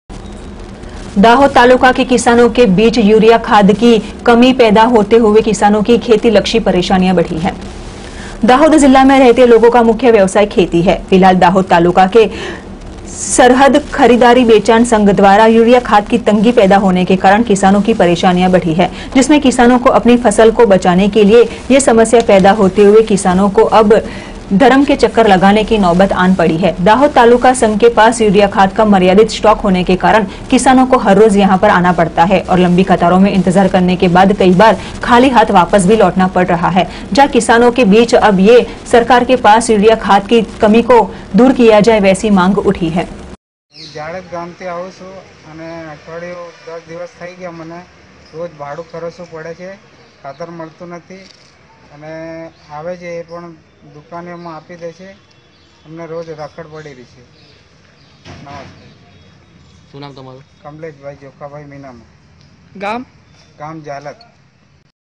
Ibn news vadodara
दाहोद तालुका के किसानों को यूरिया खाद की तंगी के कारण बड़ी परेशानी